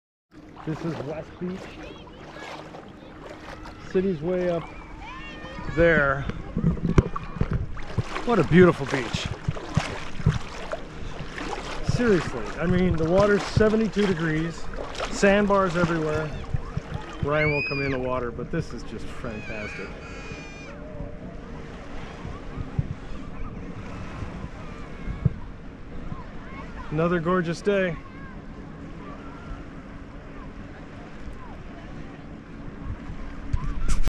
Indiana Dunes National Park - West Beach Swimming (360 VR Video)
Explore the breathtaking beauty of West Beach with crystal clear waters and sandy shores in this immersive video tour. Join us as we soak in the stunning views and serene atmosphere of this coastal paradise. Dive into the 72-degree waters and discover the hidden gems of the sandbars. Watch as Ryan enjoys the refreshing ocean waves in this fantastic beach day. Don't miss out on experiencing another gorgeous day at West Beach! Let this video transport you to a seaside getaway like no other.

Scott's in the water filming while Ryan is on the beach. This is a good look at the West Beach Lake front and swimming area. Perfect weather today with good shoreline views.

Timestamps:
1. "Explore West Beach - 00:00:00"
2. "Scenic Views of the City - 00:00:03"
3. "Beauty of the Beach - 00:00:08"
4. "Warm Waters at 72 Degrees - 00:00:11"
5. "Sand Bars Galore - 00:00:15"
6. "Ryan Takes a Dip - 00:00:17"
7. "Enjoying the Fantastic Beach - 00:00:19"
8. "Another Gorgeous Day - 00:00:26"